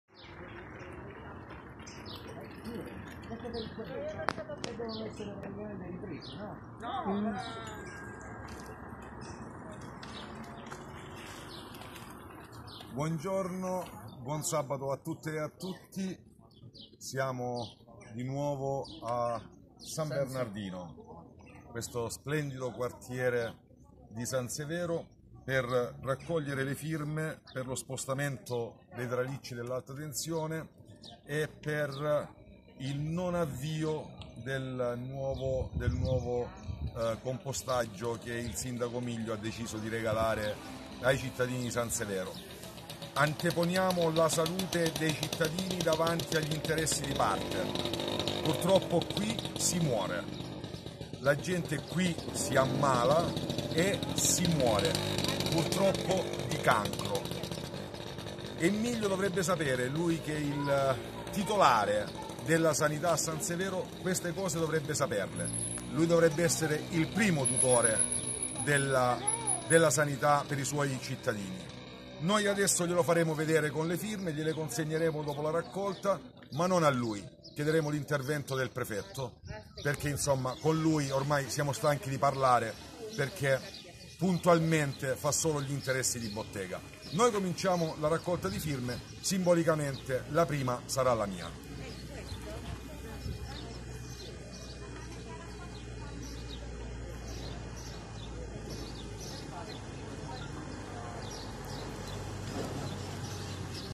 San Severo, al quartiere San Bernardino, L'ALTRA ITALIA contro il degrado che subiscono i residenti
In un contesto sociale disastrato da incuria di strade, erbacce in forte crescita, stabili fatiscenti è al limite della sicurezza, famiglie disagiate è prive di aiuto a questo aggiungiamoci i tralicci dell'alta tensione che da anni fanno bella mostra in città ma in particolar modo al quartiere San Bernardino, L'ALTRA ITALIA con la presenza dei suoi dirigenti locali continua imperterrita a offrire vicinanza, anche oggi con raccolta firme, uno dei tanti commenti espressi dal segretario nazionale Mino Cartelli, sempre presente a San Severo è :  ciò che Miglio, sindaco di questa maestosa è produttiva città non fa' L'ALTRA ITALIA pone le basi di vicinanza è fratellanza a questo quartiere, abbandonato, cancellato dalla mappa della città, in questo quartiere bisogna investire in risorse ma principalmente in Amore
Rincara la dose l'interprovinciale del movimento Siorini Saverio, frasi forte :
San Bernardino, zone case popolari, Texas un agglomerato di popolazione semplice, lavoratori, mamme che con sacrifici, spinti dall'amore della propria famiglia faticano a reggere il proprio nucleo, mariti che piangono ed evitano azioni con conseguenze tragiche..perché ? Miglio, non sono numeri che devi ricordarli durante le votazioni, amare è una parola che non conosci è non nasconderti sotto la paura di alcune frasi dette da un giovane che si è sentito l'obbligo di difendere i suoi cari...MIGLIO abbi il buon senso di responsabilità di dimetterti